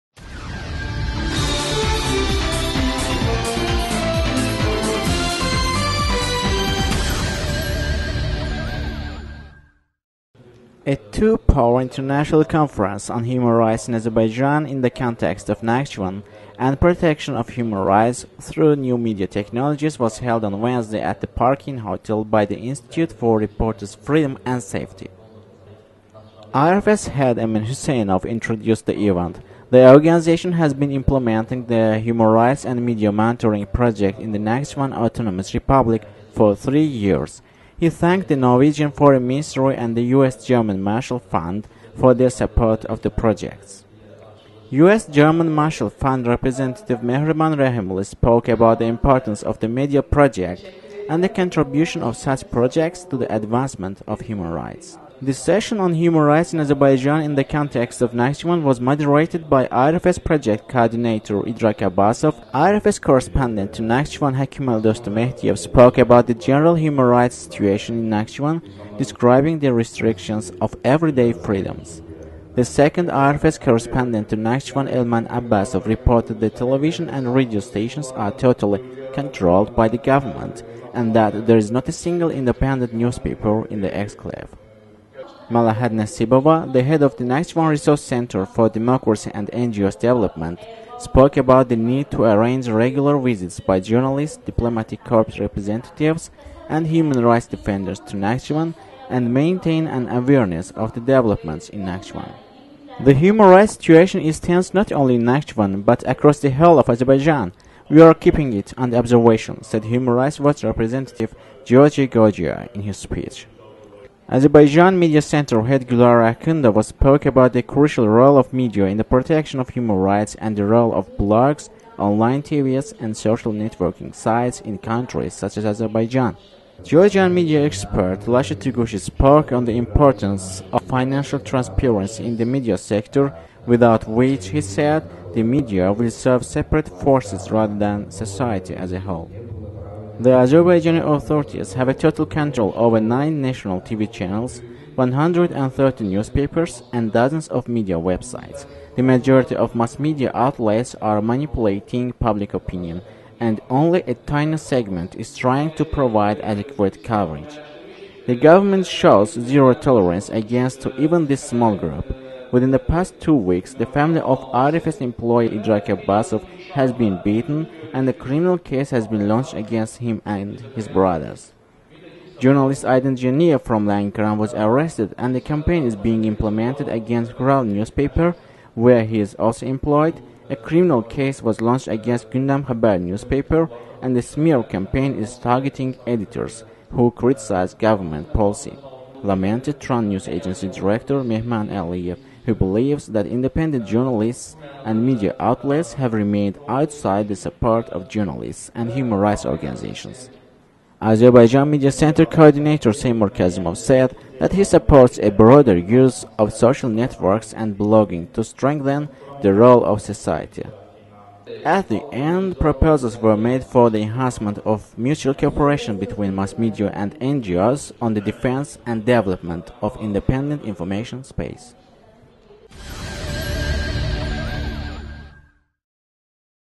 INTERNATIONAL CONFERENCE ON HUMAN RIGHTS LOOKS AT NAKHCHIVAN AND NEW MEDIA TECHONOLOGIES
A two part international conference on "Human Rights in Azerbaijan in the context of Nakhchivan" and "Protection of human rights through new media technologies" was held on Wednesday at the Park Inn Hotel by the Institute for Reporters' Freedom and Safety.

IRFS head Emin Huseynov introduced the event. The organization has been implementing the Human Rights and Media Monitoring project in the Nakhchivan Autonomous Republic for 3 years. He thanked the Norwegian Foreign Ministry and the US German Marshall Fund for their support of the projects.

US German Marshall Fund representative Mehriban Rahimli spoke about the importance of the media project and the contribution of such projects to the advancement of human rights.

The session on "Human Rights in Azerbaijan in the context of Nakhchivan" was moderated by IRFS project coordinator Idrak Abbasov. IRFS correspondent to Nakhchivan Hakimeldostu Mehdiyev spoke about the general human rights situation in Nakhchivan, describing the restrictions of every day freedoms. The second IRFS correspondent to Nakhchivan, Elman Abbasov, reported that the television and radio stations are totally controlled by the government, and that there is not a single independent newspaper in the exclave.

Malahat Nasibova, head of the Nakhchivan Resource Center for Democracy and NGOs Development, spoke about the need to arrange regular visits by journalists, diplomatic corps representatives and human rights defenders, and maintain an awareness of the developments in Nakhchivan.

"The human rights situation is tense not only in Nakhchivan but across the whole of Azerbaijan. We are keeping it under observation," said Human Rights Watch representative Georgi Gogia in his speech.

Azerbaijan Media Center Head Gulnara Akhundova spoke about the crucial role of media in the protection of human rights and the role of blogs, online TVs and social networking sites in countries such as Azerbaijan.

Georgian media expert Georgia Lasha Tugushi spoke on the importance of financial transparency in the media sector, without which, he said, the media will serve separate forces rather than society as a whole.

"The Azerbaijani authorities have a total control over 9 national TV channels, 130 newspapers and dozens of media websites. The majority of mass media outlets are manipulating public opinion, and only a tiny segment is trying to provide adequate coverage. The government shows zero tolerance against to even this small group. Within the past two weeks, the family of IRFS employee Idrak Abbasov has been beaten, and a criminal case has been launched against him and his brothers. Journalist Aydin Janiyev from Lankaran was arrested and a campaign is being implemented against Khural newspaper, where he is also employed, a criminal case was launched against Gundam Khabar newspaper and a smear campaign is targeting editors who criticize governemnt policy," lamented Turan news agency director Mehman Aliyev, who believes that independent journalists and media outlets have remained outside the support of journalists and human rights organizations.

Azerbaijan Media Center Coordinator Seymur Kaizmov said that he supports a broader use of social networks and blogging to strengthen the role of civil society.

At the end, proposals were made for the enhancement of mutual cooperation between mass media and NGOs on the defence and development of independent information space.